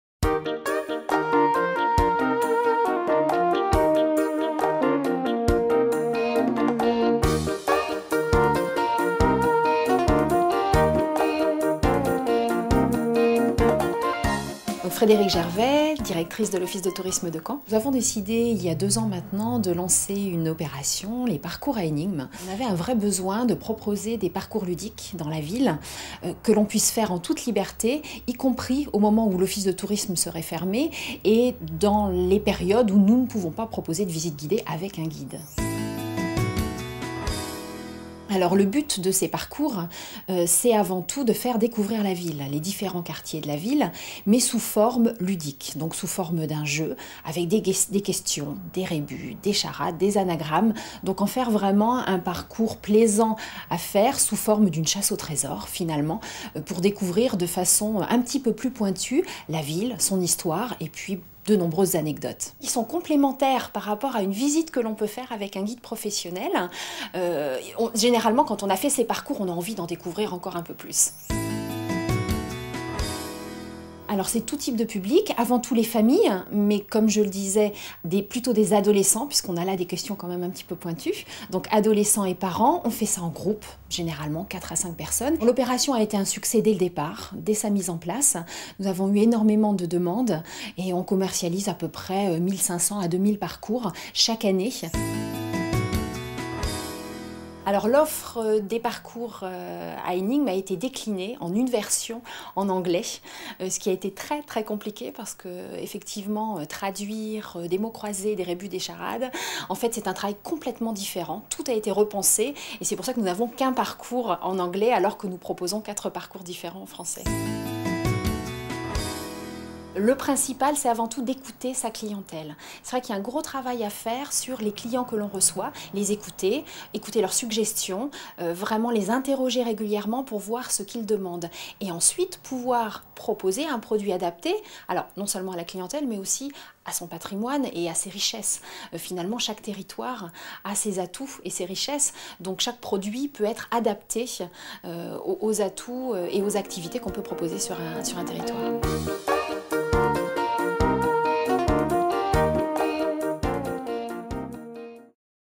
Office de Tourisme de Caen - Trophées du Tourisme 2010 du Calvados
L'Office de Tourisme de Caen a été récompensé par les Trophées du Tourisme du Calvados en 2010 pour les rallyes touristiques "Caen de la tête aux pieds".